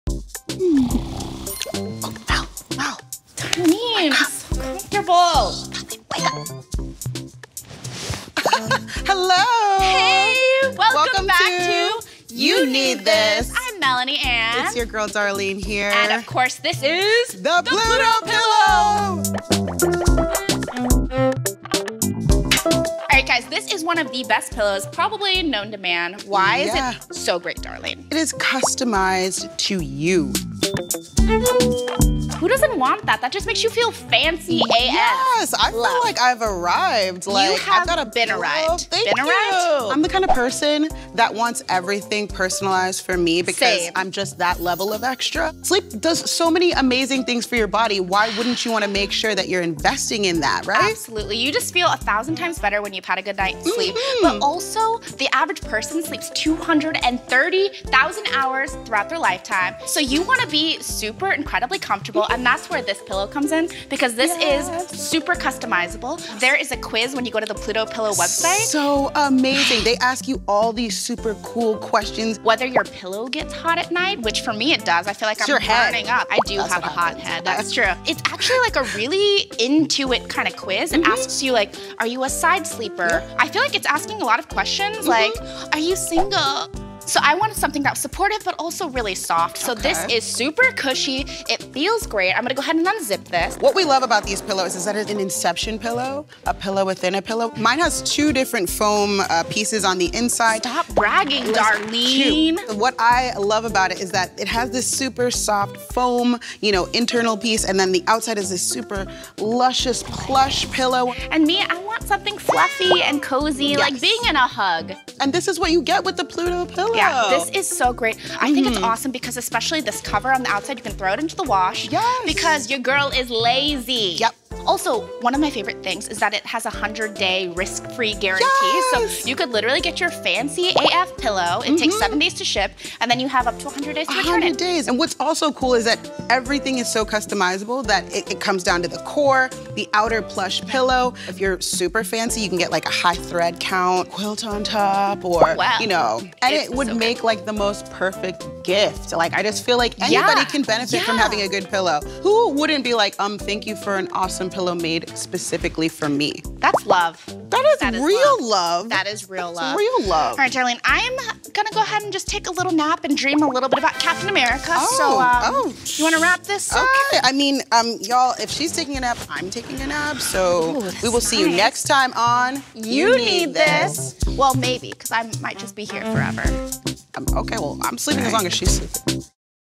The customizable Pluto pillow will give you the best sleep of your life
This pillow is fit just for you and all your sleeping needs! All you have to do is take a quiz online to get the best snooze ever.

Our team is dedicated to finding and telling you more about the products and deals we love. Pricing and availability are subject to change.